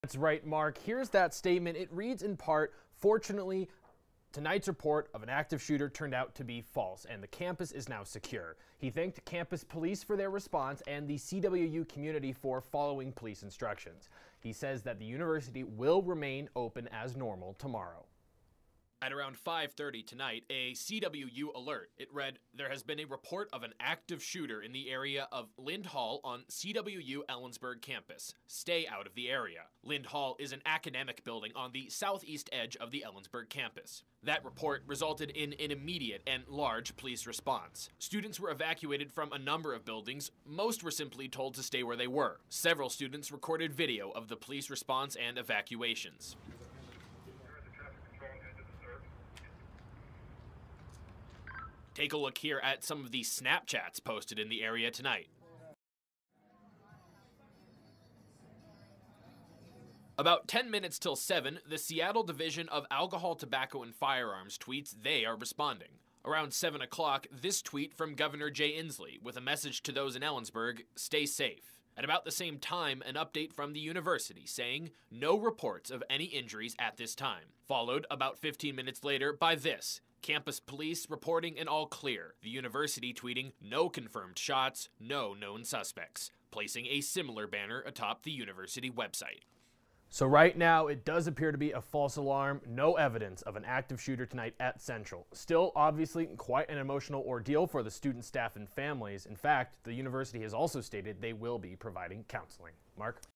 Central Washington University gives all-clear after active shooter report
An active shooter was reported at Central Washington University in Ellensburg on Wednesday night, but no injuries were reported.